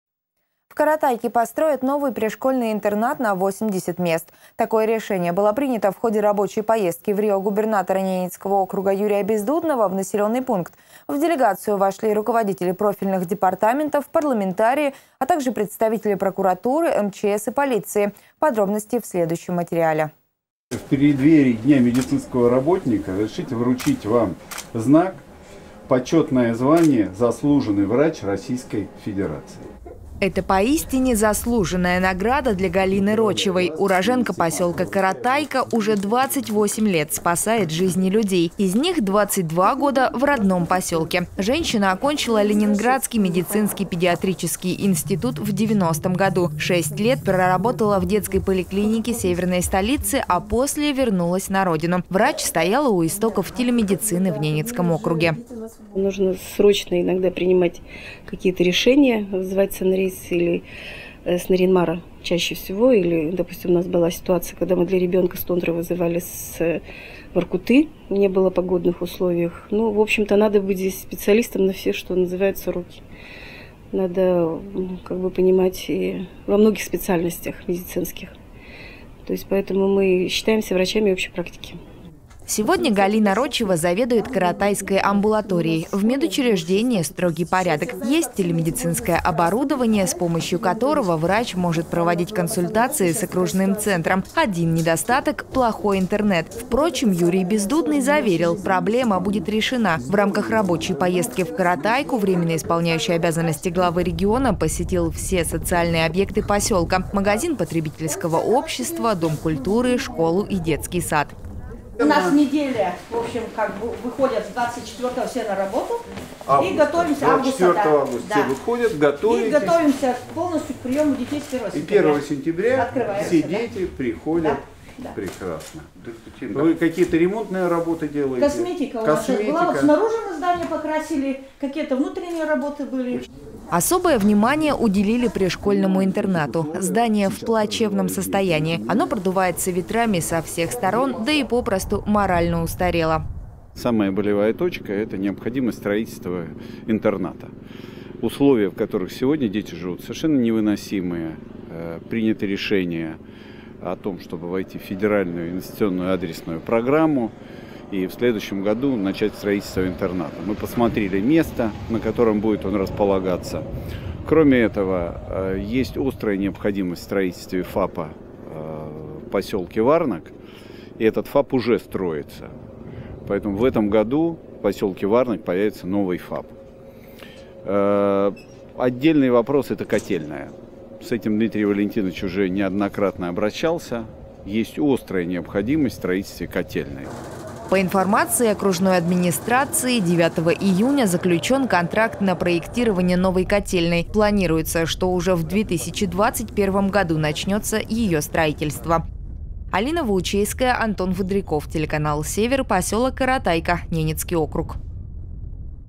ЧЕМ ЖИВЕШЬ, КАРАТАЙКА?
В Каратайке построят новый пришкольный интернат на 80 мест. Такое решение было принято в ходе рабочей поездки ВРИО губернатора Ненецкого округа Юрия Бездудного в населенный пункт. В делегацию вошли руководители профильных департаментов, парламентарии, а также представители прокуратуры, МЧС и полиции.